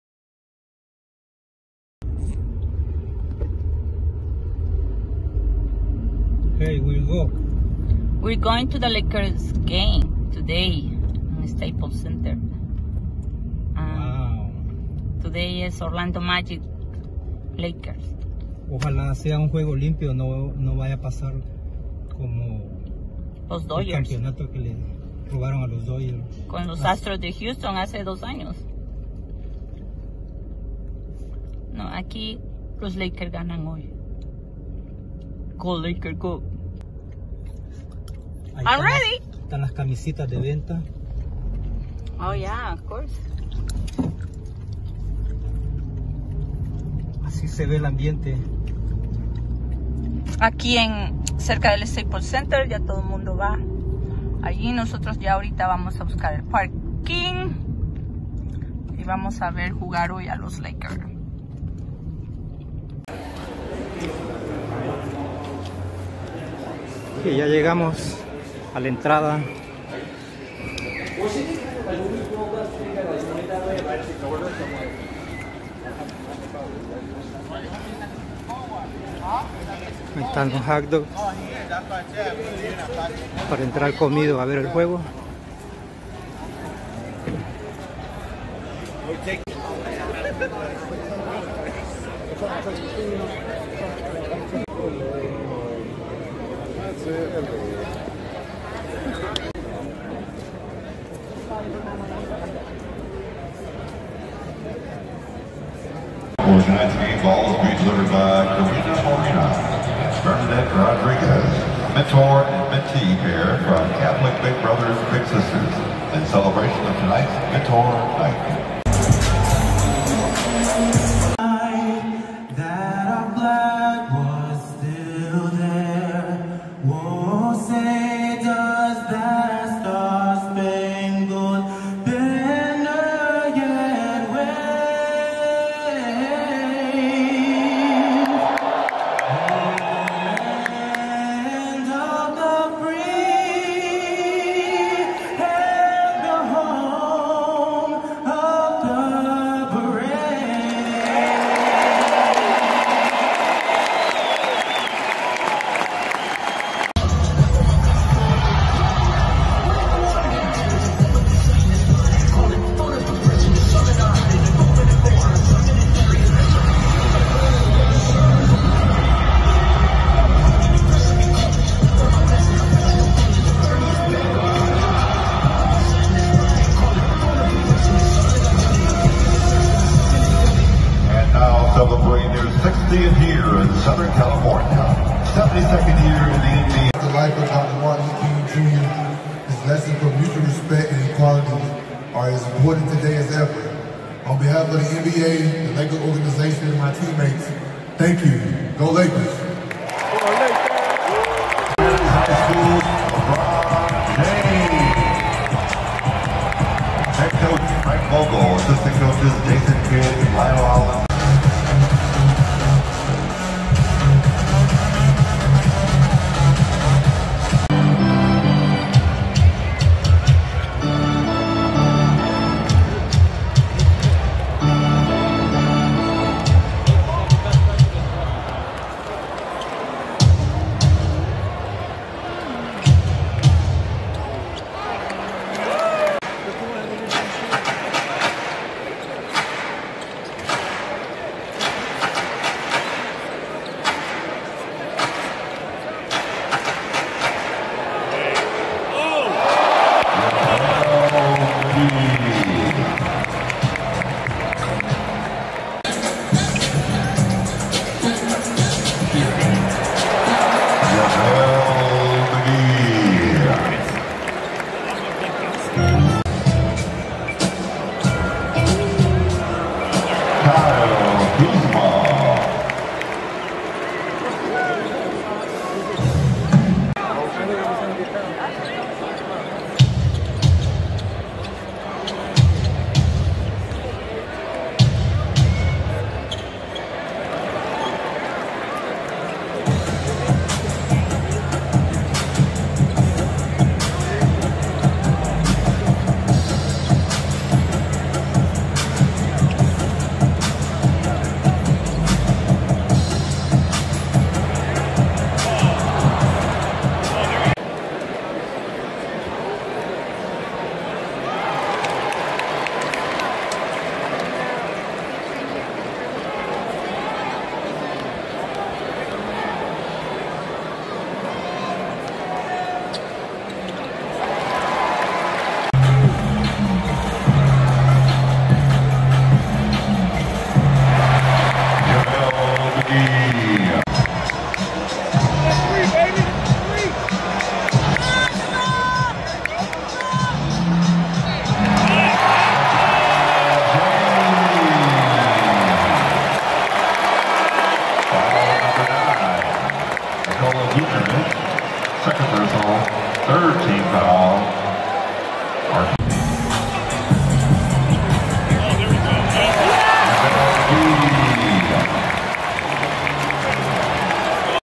Ángeles Lakers VS Orlando Magic January 2020
Nos fuimos al Staple  Center  Downtonwn los Ángeles CA miércoles 15 enero ,fue un gran juego pero perdimos Orlando 119 Lakers 118😥